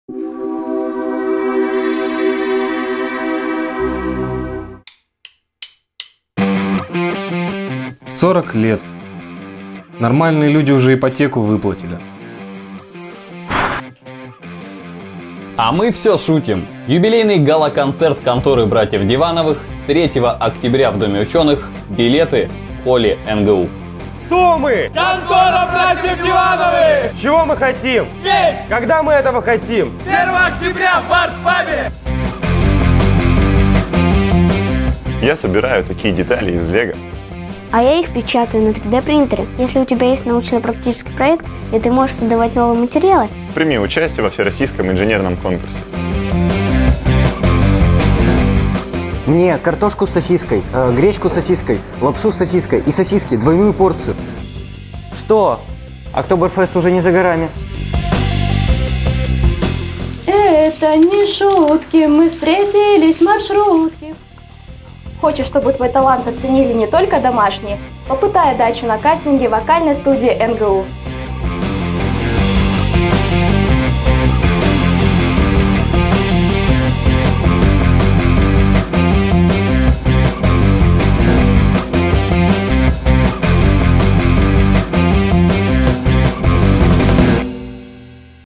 NSU LIFE АФИША. 28 сентября — 4 октября
У Конторы братьев Дивановых юбилей, им 40 лет! Об этом и о многом другом — в нашей Афише.